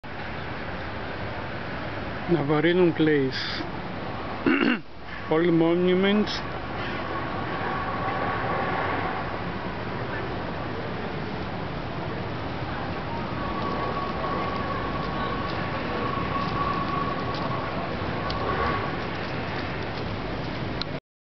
Navarino square -ancient monuments
a quick view of the ancient monuments of Navarino square .Taken with photo camera (= little unstable )